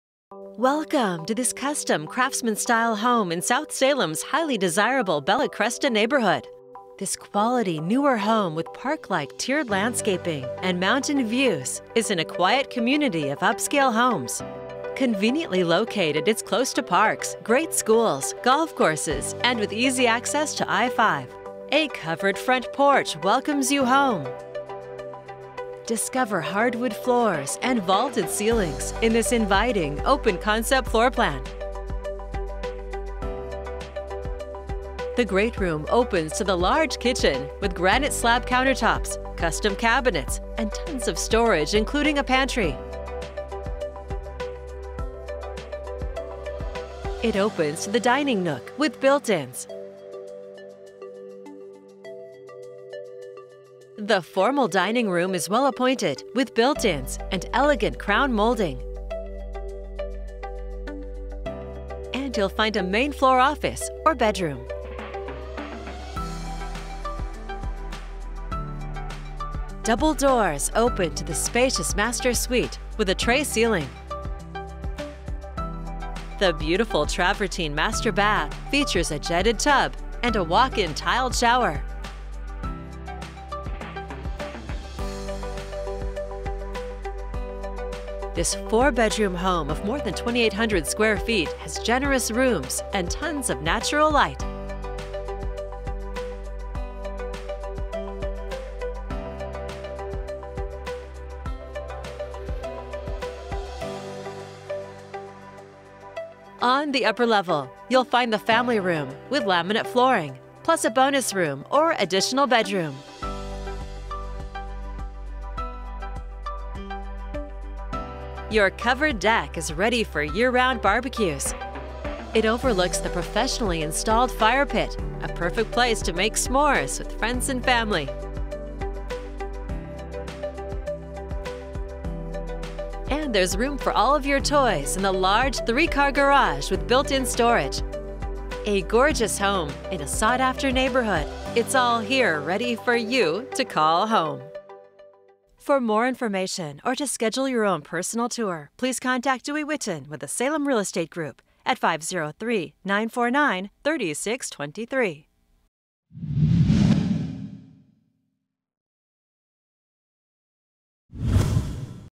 Beautiful Custom Craftsman Home in South Salem ~ Video of 2343 Milano Ave.
Custom Craftsman with gorgeous high end finishes in upscale South Salem neighborhood. Kitchen with slab granite counters, trex deck, master suite with travertine master bath, professionally installed fire pit, mountain views! Contact Dewey Whitton with the Salem Real Estate Group.